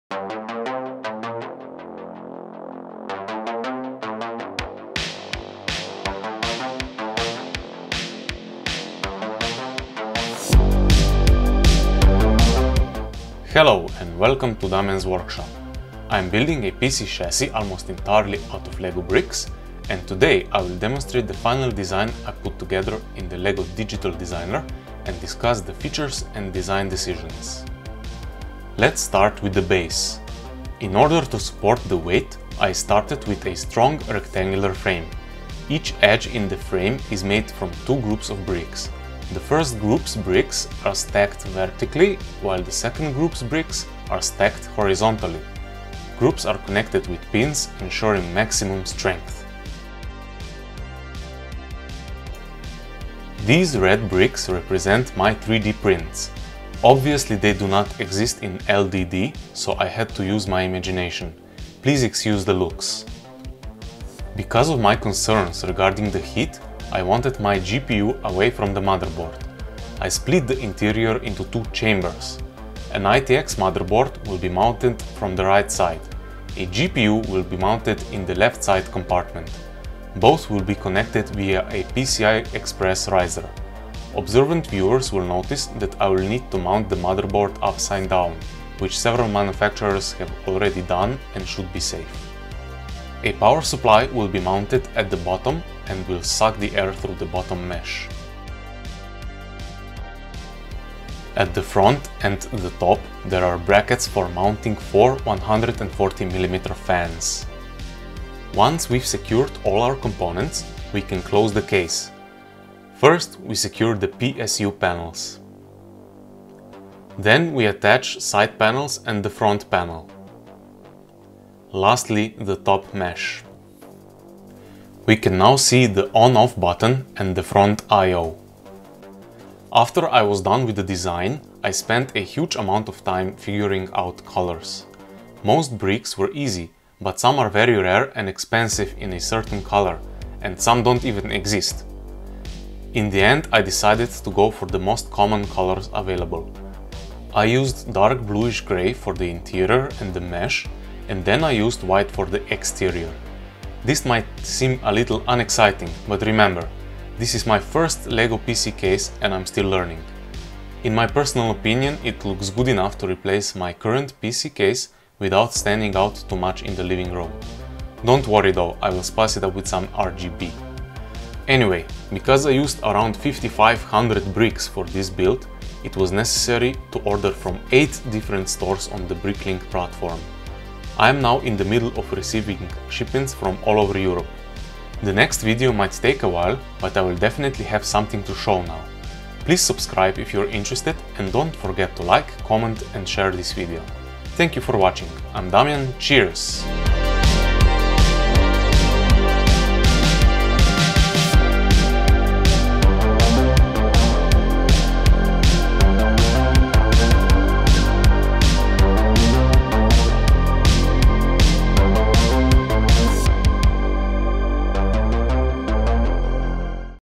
LEGO PC Case Part 8.4: The Design Summary
In this video I explain features and design decisions for my PC Case.

I am building a custom LEGO computer case. To mount PC components to LEGO bricks, I require special 3D printed parts.